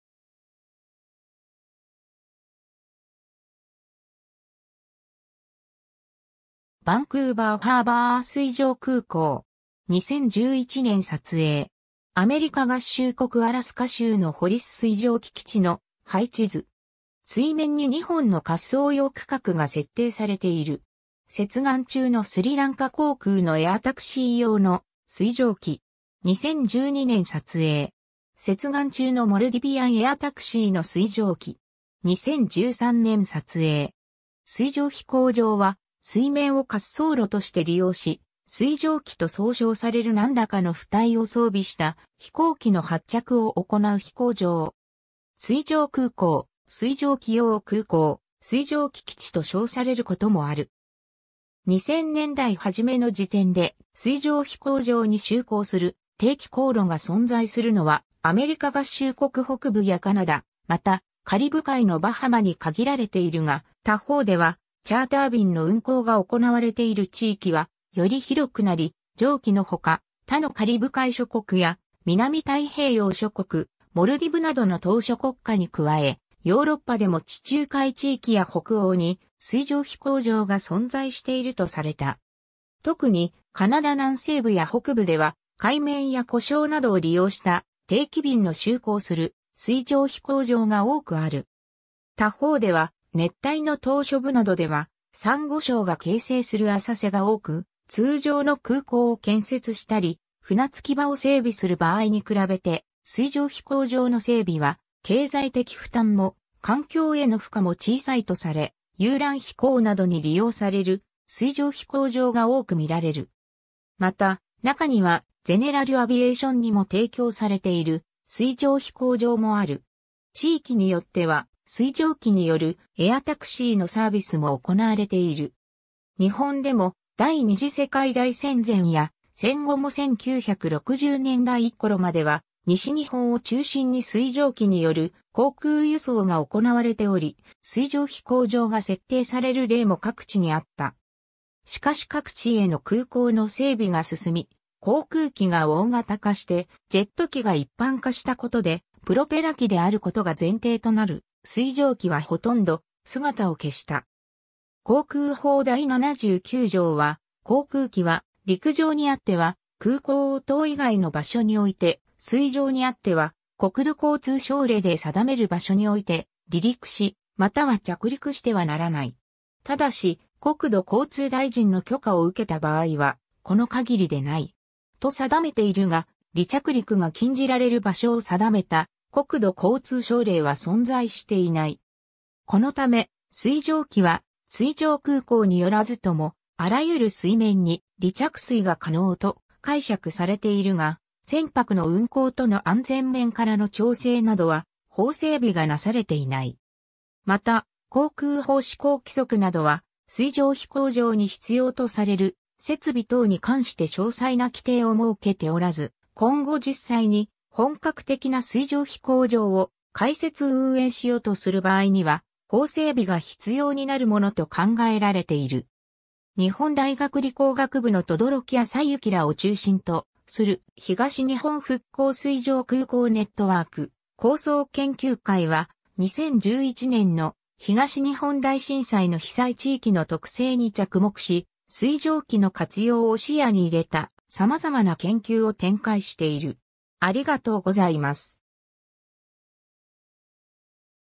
水上飛行場
バンクーバー・ハーバー水上空港。
2011年撮影。
 アメリカ合衆国アラスカ州のホリス水上機基地の配置図。
水面に2本の滑走用区画が設定されている。
 接岸中のスリランカ航空のエアタクシー用の水上機。
2012年撮影。
 接岸中のモルディビアン・エアタクシーの水上機。
2013年撮影。
 水上飛行場（すいじょうひこうじょう、英語: Water aerodrome）は、水面を滑走路として利用し、水上機と総称される何らかの浮体を装備した飛行機の発着を行う飛行場。
水上空港（すいじょうくうこう）、水上機用空港（すいじょうきようくうこう、英語: Water Airport）、水上機基地（すいじょうききち、英語: Seaplane base）と称されることもある。
 2000年代はじめの時点で、水上飛行場に就航する定期航路が存在するのは、アメリカ合衆国北部やカナダ、また、カリブ海のバハマに限られているが、他方ではチャーター便の運航が行われている地域はより広くなり、上記のほか、他のカリブ海諸国や、南太平洋諸国、モルディブなどの島嶼国家に加え、ヨーロッパでも地中海地域や北欧に水上飛行場が存在しているとされた。
特にカナダ南西部や北部では、海面や湖沼などを利用した、定期便の就航する水上飛行場が多くある。
他方では熱帯の島嶼部などでは、珊瑚礁が形成する浅瀬が多く、通常の空港を建設したり、船着場を整備する場合に比べて水上飛行場の整備は、経済的負担も環境への負荷も小さいとされ、遊覧飛行などに利用される水上飛行場が多く見られる。
また、中にはゼネラル・アビエーションにも提供されている水上飛行場もある。
 地域によっては、水上機によるエアタクシーのサービスもおこなわれている。
 日本でも、第二次世界大戦前や、戦後も1960年代ころまでは、西日本を中心に水上機による航空輸送がおこなわれており、水上飛行場が設定される例も各地にあった。
しかし各地への空港の整備が進み、航空機が大型化してジェット機が一般化したことで、プロペラ機であることが前提となる水上機はほとんど姿を消した。
 航空法第七十九条は、「航空機（国土交通省令で定める航空機を除く。）は、陸上にあつては空港等以外の場所において、水上にあつては国土交通省令で定める場所において、離陸し、又は着陸してはならない。ただし、国土交通大臣の許可を受けた場合は、この限りでない。」と定めているが、離着陸が禁じられる場所を定めた国土交通省令は存在していない。
このため、水上機は水上空港によらずとも、あらゆる水面に離着水が可能と解釈されているが、船舶の運航との安全面からの調整などは、法整備がなされていない。
 また、航空法施行規則などは、水上飛行場に必要とされる設備等に関して詳細な規定を設けておらず、今後実際に本格的な水上飛行場を開設運営しようとする場合には、法整備が必要になるものと考えられている。
 日本大学理工学部の轟朝幸らを中心とする「東日本復興水上空港ネットワーク構想研究会」は、2011年の東日本大震災の被災地域の特性に着目し、水上機の活用を視野に入れた様々な研究を展開している。